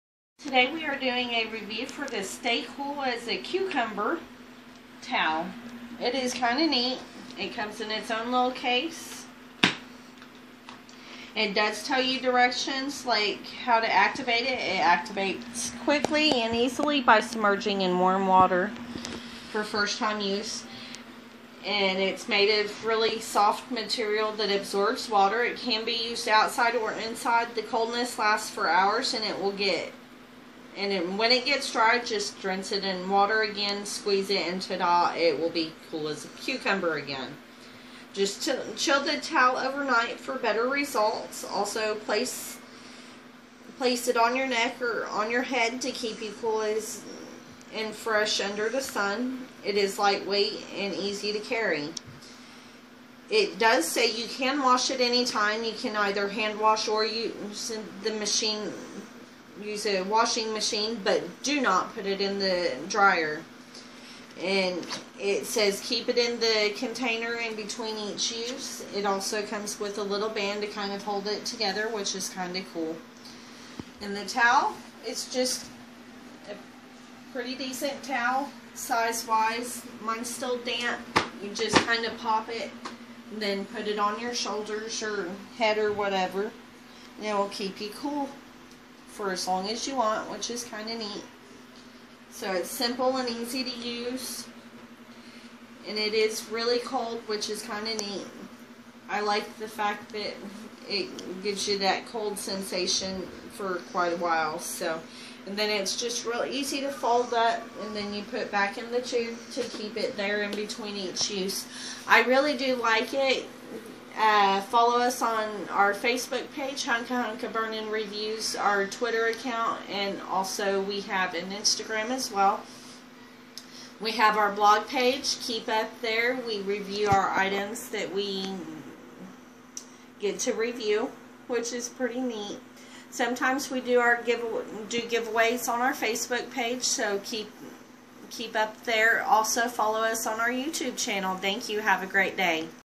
Cool as a cucumber towel
This towel is pretty neat! Just get it wet wring it out and use it! It helps you to stay cool! I am liking it! If you think you would like to check out and get yourself one on Amazon